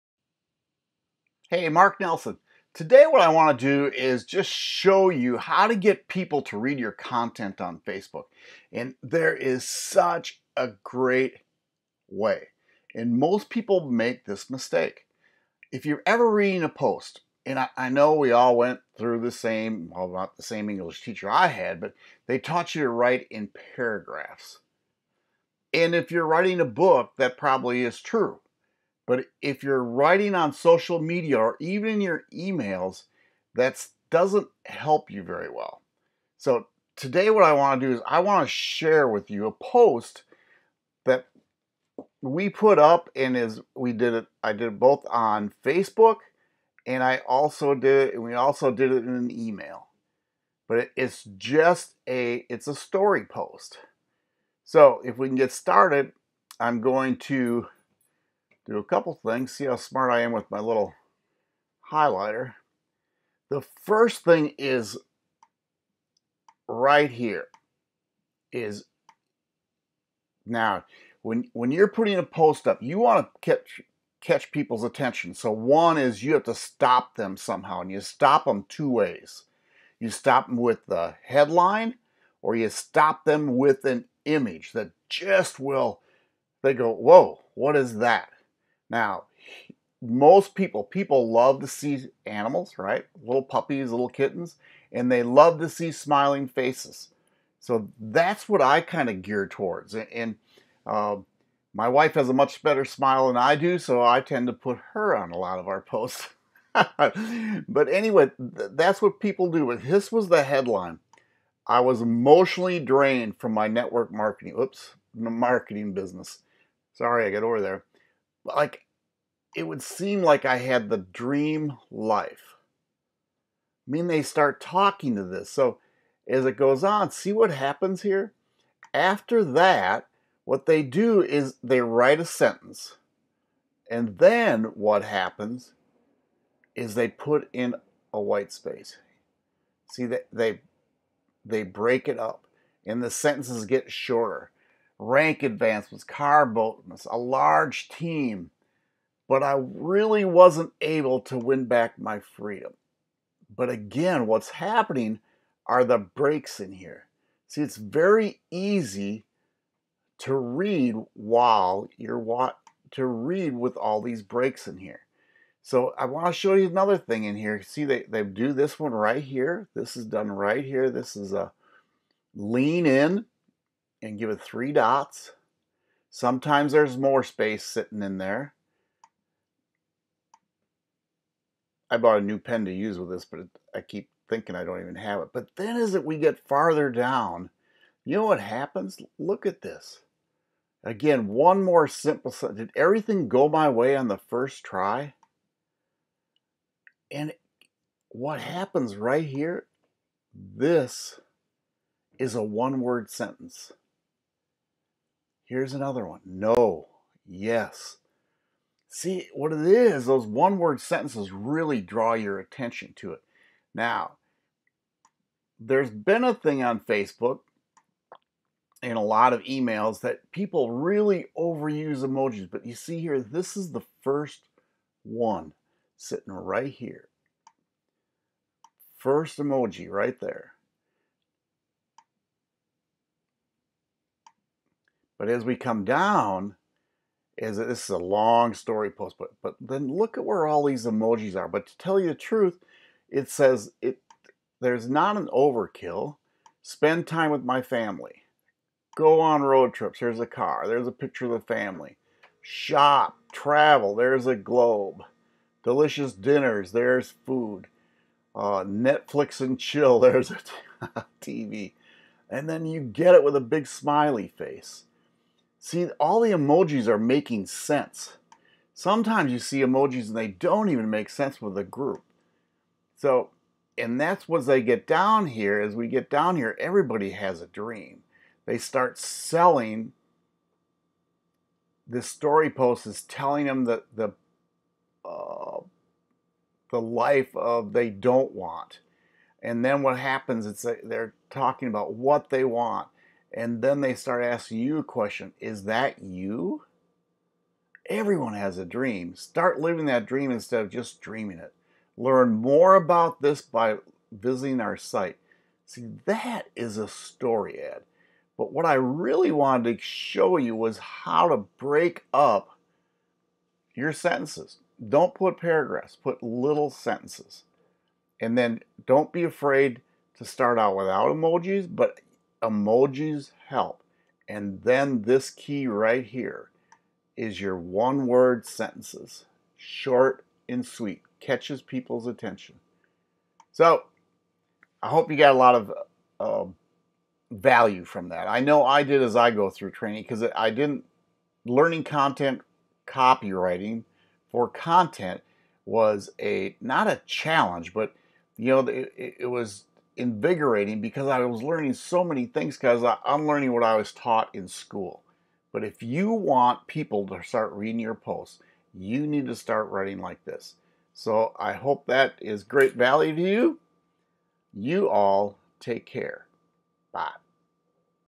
How To Get People To Read Your Content On Facebook
In this video, I share how to get people to read your content on Faceboook